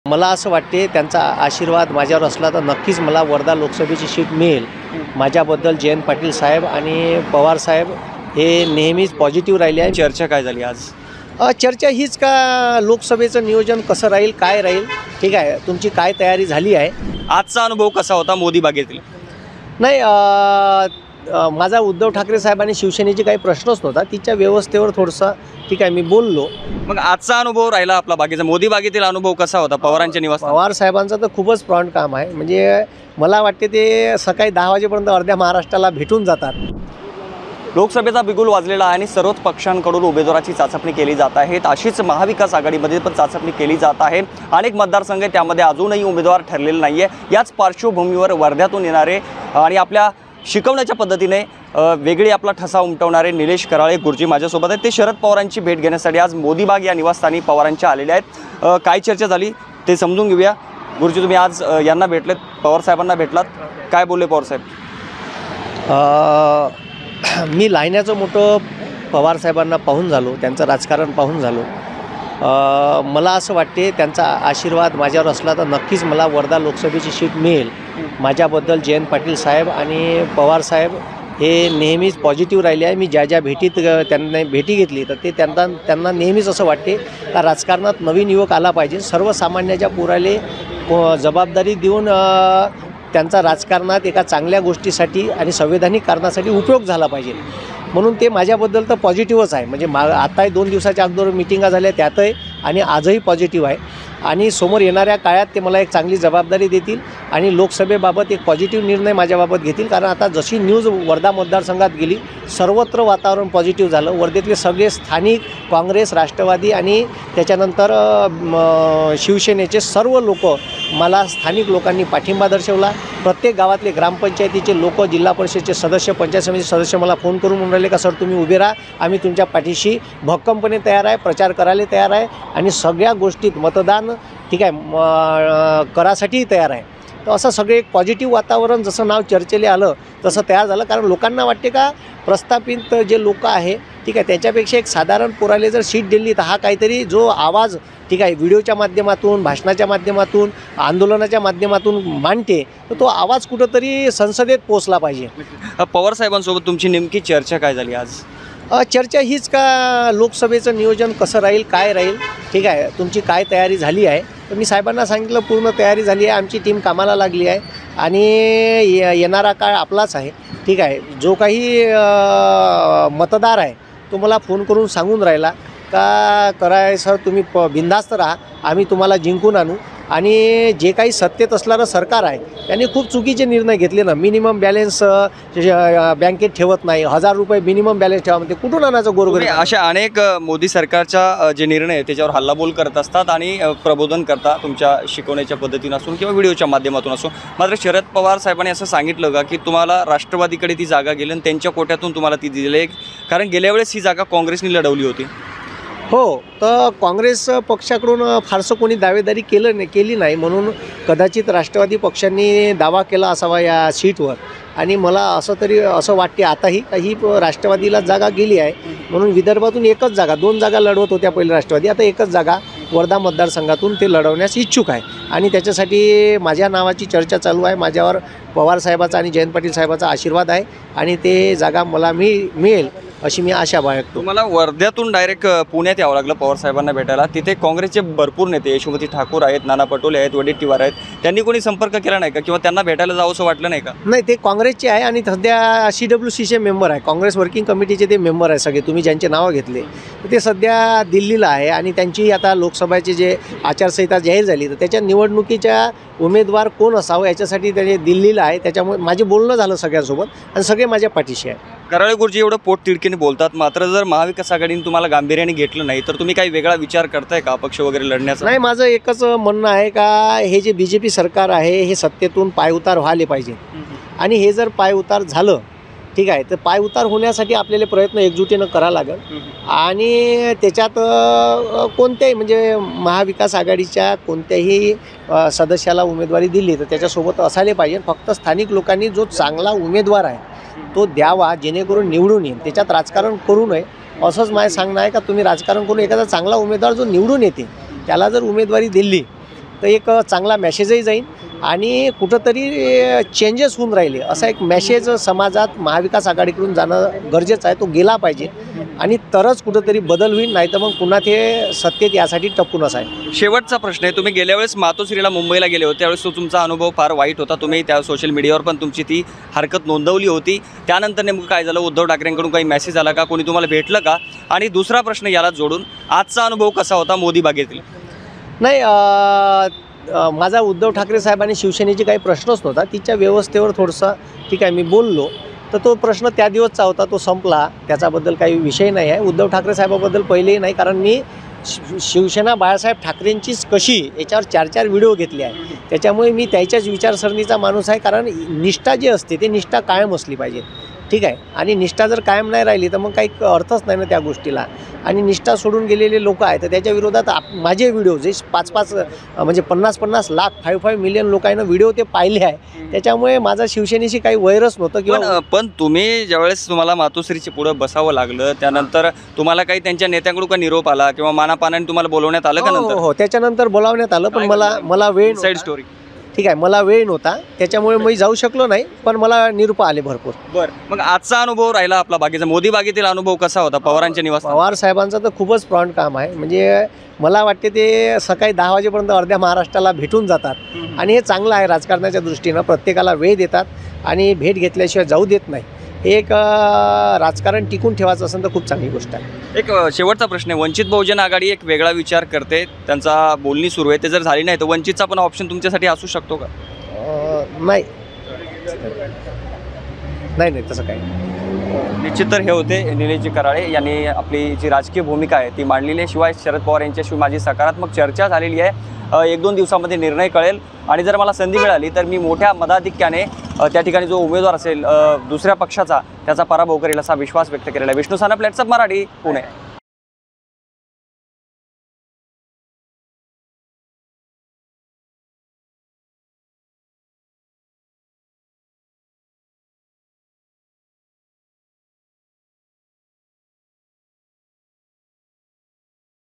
Nitesh Karale Sir : वर्धा लोकसभा : तयारीला लागा... कराळे गुरुजींना पवारांची सूचना | LetsUpp Marathi
वर्धा लोकसभेसाठी इच्छुक नितेश कराळे यांनी शरद पवारांची भेट घेतली. या पार्श्वभूमीवर लेट्सअपने त्यांच्याशी संवाद साधत त्यांच्या भावना जाणून घेतल्या.